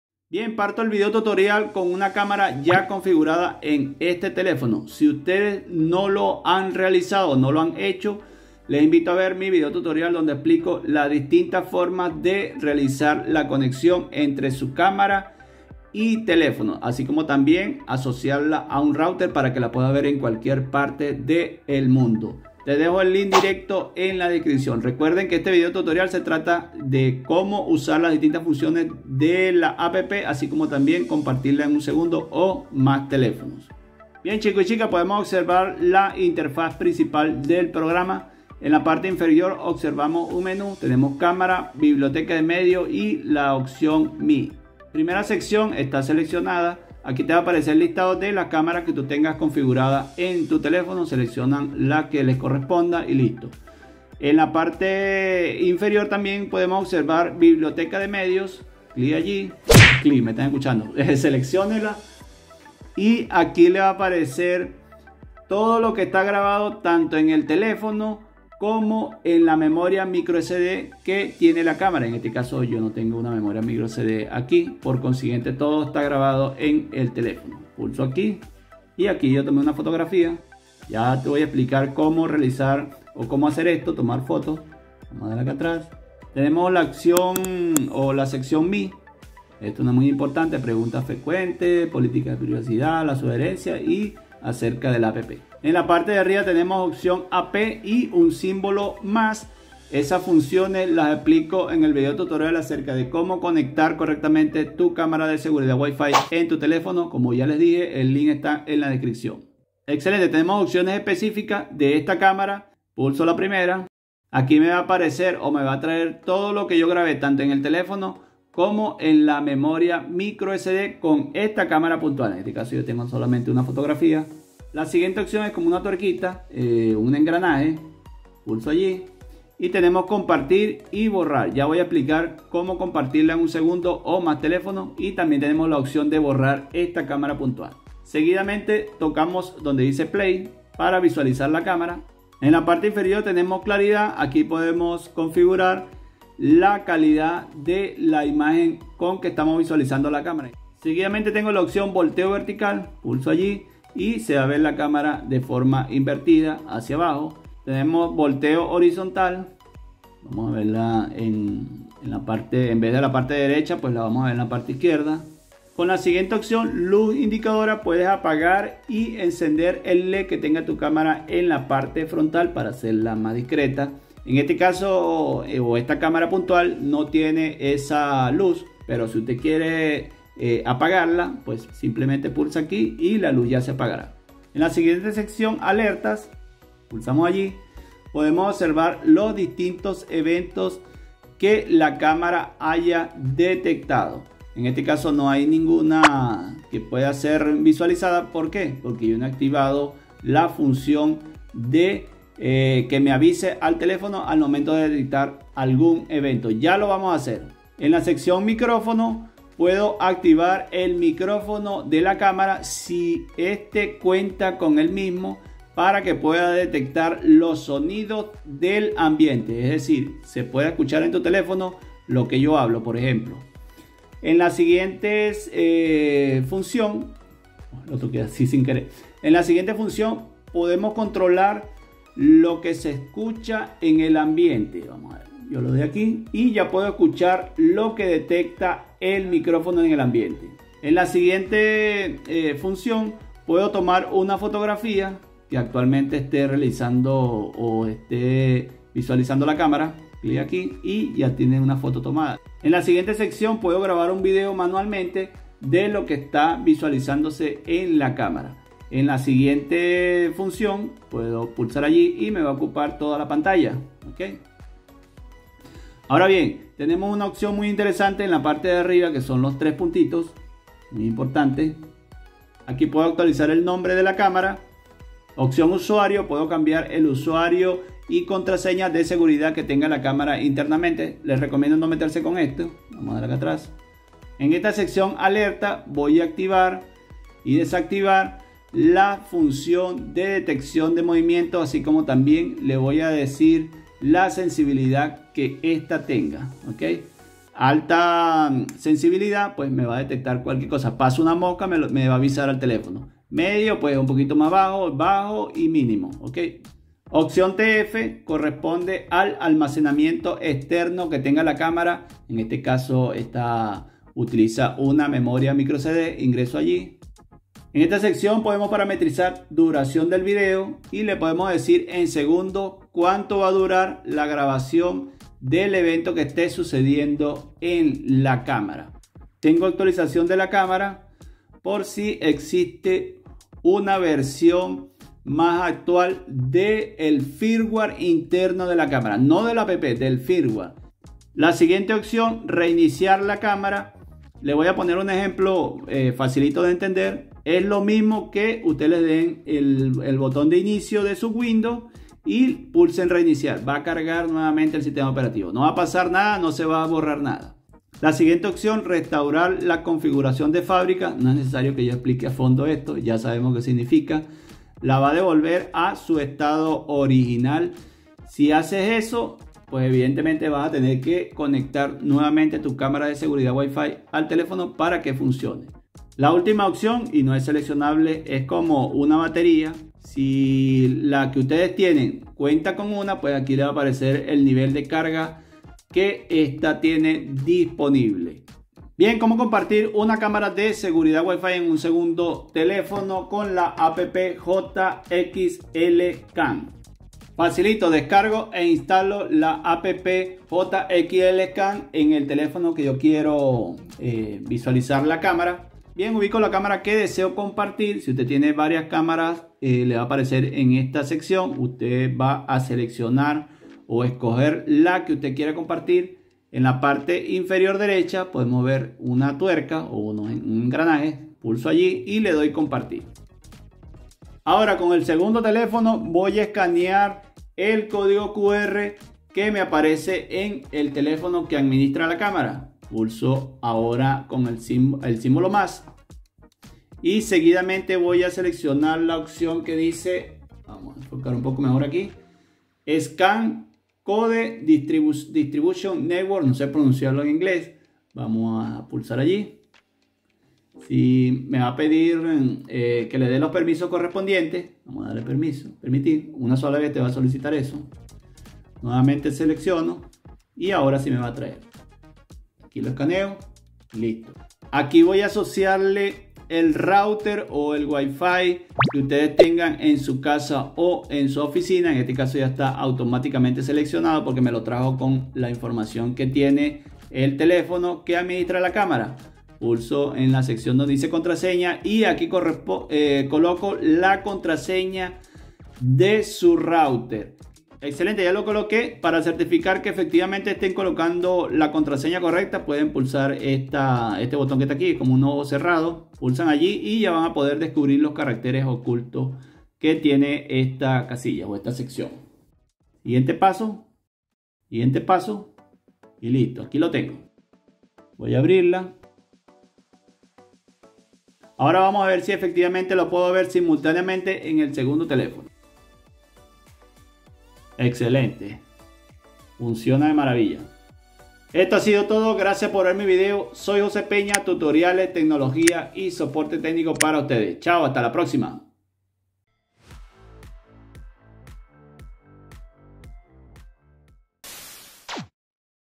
Como Compartir Cámara de Seguridad WiFi en Varios Teléfonos con JXLCAM👈
👉EXPLICADO CON LA NUEVA VERSIÓN DE JXLCAM👈, ¡Véalo AHORA!.
En éste vídeo explico de forma sencilla como usar la aplicacion JXLCAM con la que puedes ver tu cámara de seguridad WiFi con tu Smartphone, teléfono Android y iPhone, asi como tambien compartirla en varios dispositivos.
Conectaremos la cámara tipo espiá, pero este video tutorial aplica para las distintas cámaras compatibles con esta APP.
Videos Cortos 👆Tecnológicos que te harán la vida más fácil.
Link´s de interés:
SI TE FALLA EL PROCESO DE CONEXIÓN AQUÍ DOS VIDEO QUE PUEDES CONSULTAR:
Capítulos:
00:00 Introducción.
00:34 Como usar la APP JXLCAM
06:29 Como compartir la cámara.
08:27 Pruebas finales.
👉👉👉👉Página Web Oficial del Canal👈👈👈👈